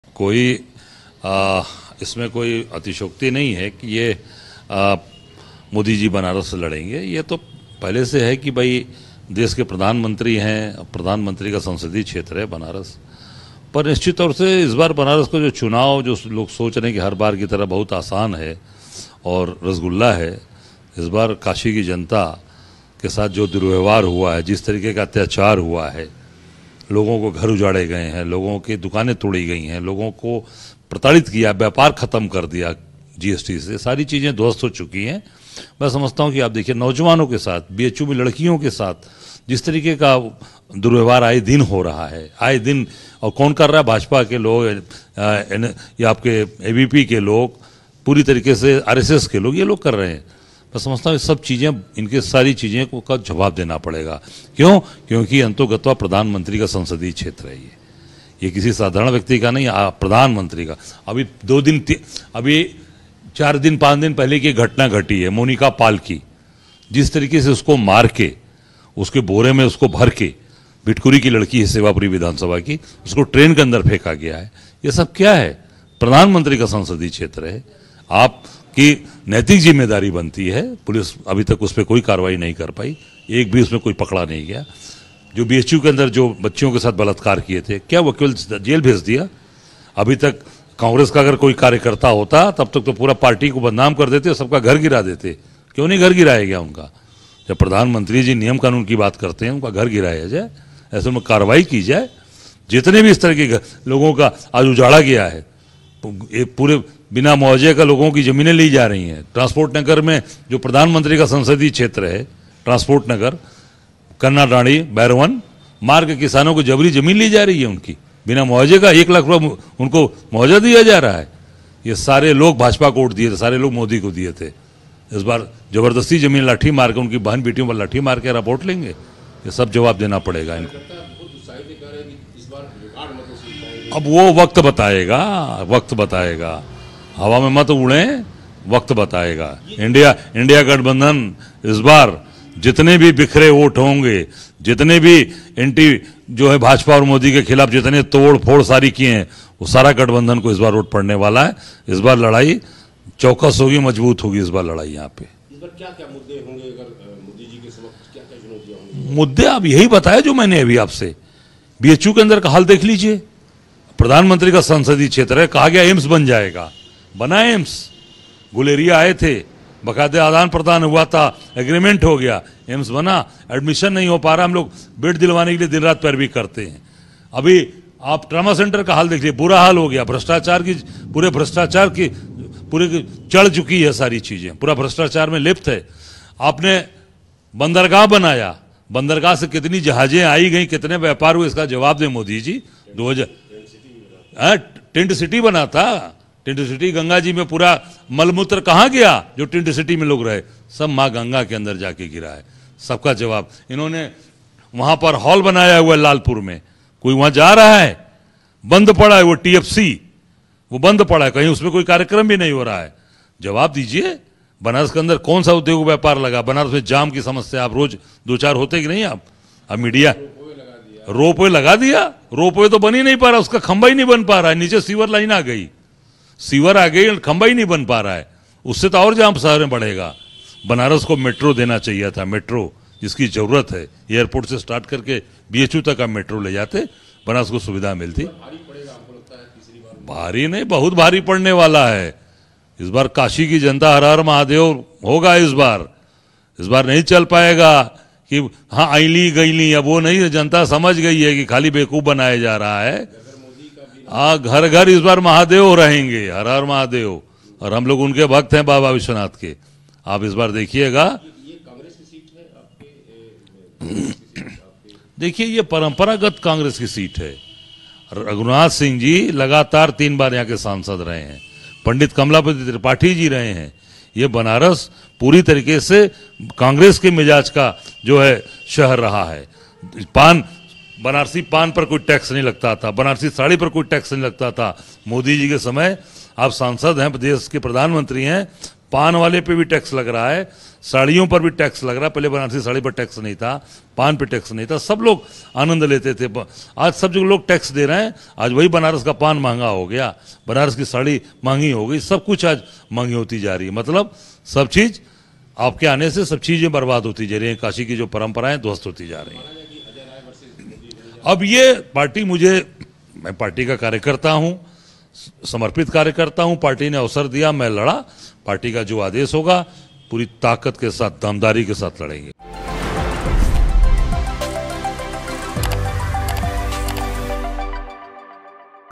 पीएम मोदी के वाराणसी से चुनाव लड़ने पर बरसे अजय राय, कांग्रेस नेता से BHU से AIIMS तक सब गिना दिया
पीएम के संसदीय क्षेत्र वाराणसी से पीएम मोदी को फिरसे प्रत्याशी बनाए जाने पर बरसे अजय राय। यूपी कांग्रेस प्रदेश अध्यक्ष ने कहा कि इस बार काशी का चुनाव भारी पड़ने वाला है क्योंकि जनता को जवाब देना होगा कि सुविधाओं के नाम पर वाराणसी को क्या मिला।